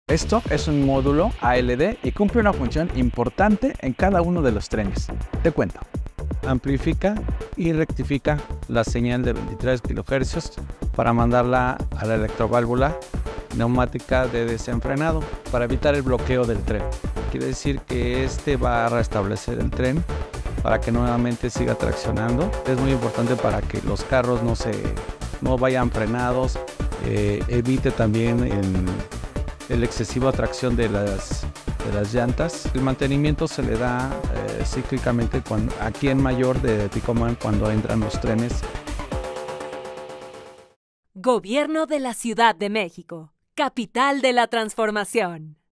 El módulo ALD está en todos los trenes y evita el bloqueo de frenado y la excesiva tracción del tren
El módulo ALD está en todos los trenes y evita el bloqueo de frenado de un tren y la excesiva tracción en la llantas. Cada carro tiene un módulo ALD y se le da mantenimiento cíclico para su óptimo desempeño en los trenes. ¡En el Metro seguimos trabajando!